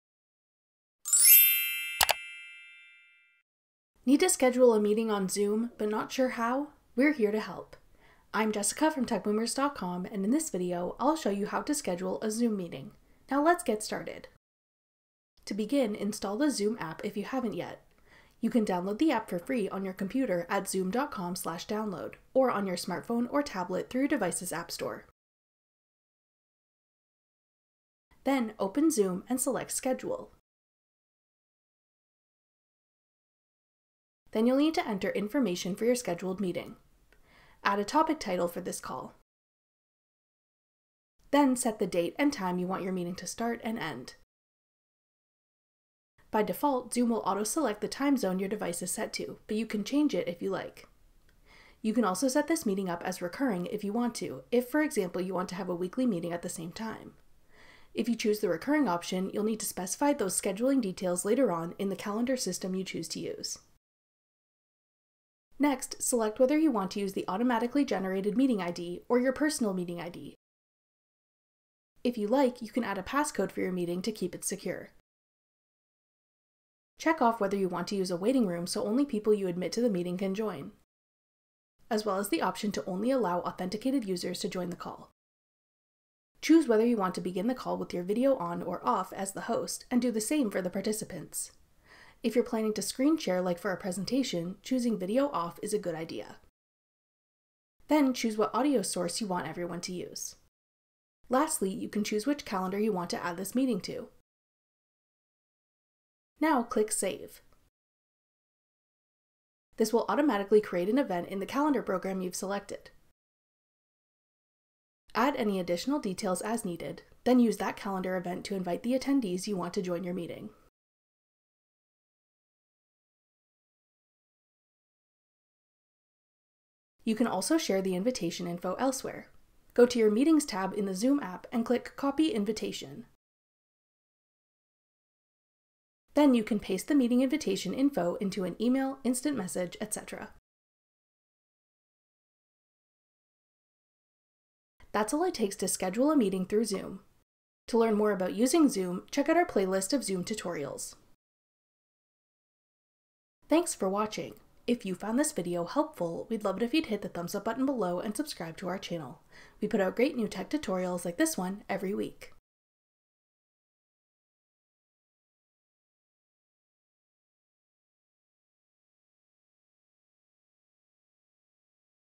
How to Schedule a Zoom Meeting
Need to schedule a meeting on Zoom but not sure how? We’re here to help. Watch this video to learn how to schedule zoom calls!

To begin, install the Zoom app if you haven’t yet.

Then, open Zoom and select Schedule. Then you’ll need to enter information for your scheduled meeting. Add a topic title for this call. Then set the date and time you want your meeting to start and end. By default Zoom will auto-select the time zone your device is set to, but you can change it if you like. You can also set this meeting up as recurring if you like, if for example you want to have a weekly meeting at the same time. If you choose the recurring option, you will need to specify those scheduling details later on in the calendar system you choose to use.

Next, select whether you want to use the automatically generated meeting ID, or your personal meeting ID. If you like, you can add a passcode for your meeting to keep it secure. Check off whether you want to use a waiting room so only people you admit to the meeting can join, as well as the option to only allow authenticated users to join the call. Choose whether you want to begin the call with your video on or off as the host, and do the same for the participants. If you’re planning to screen share like for a presentation, choosing video off is a good idea. Then, choose what audio source you want to use. Lastly you can choose which calendar you want to add this meeting to. Now click Save. This will automatically create an event in the calendar program you’ve selected. Add any additional details as needed, then use that calendar event to invite the attendees you want to join your meeting. You can also share the invitation info elsewhere. Go to your Meetings tab in the Zoom app and click Copy Invitation. Then you can paste the meeting invitation info into an email, instant message, etc.